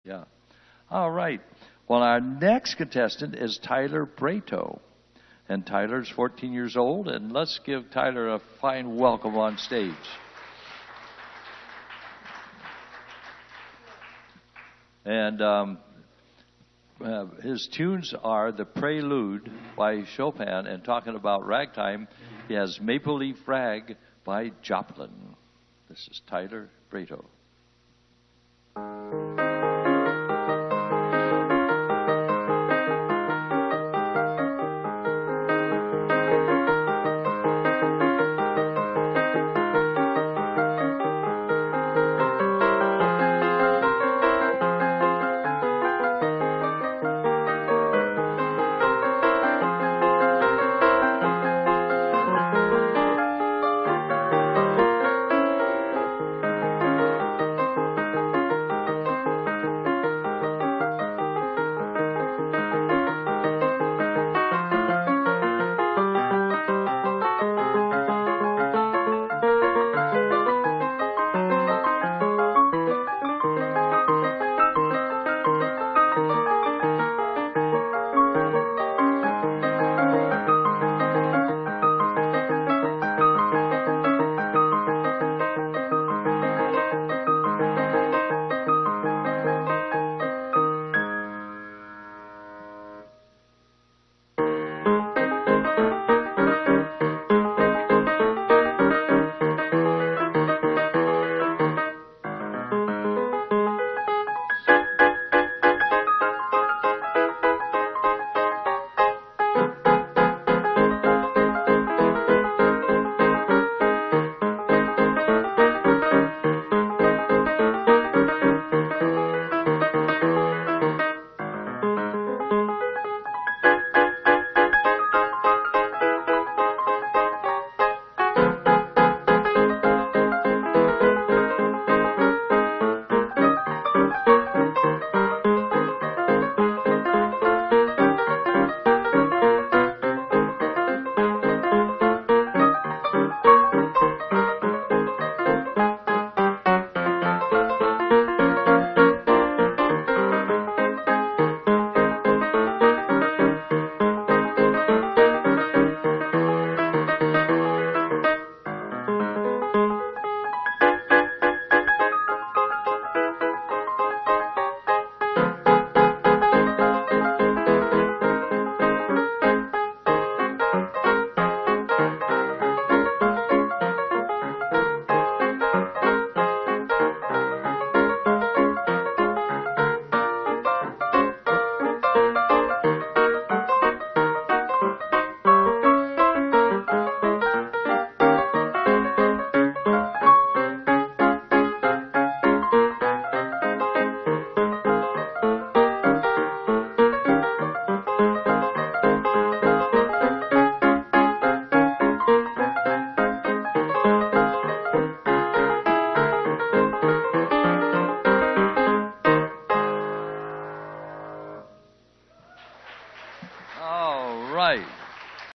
2015-10-24 Piano Contest - Tyler - 2015 Western Open Fiddle Championships
33rd Annual Western Open Fiddle Championships - October 23-24, 2015 - Red Bluff, CA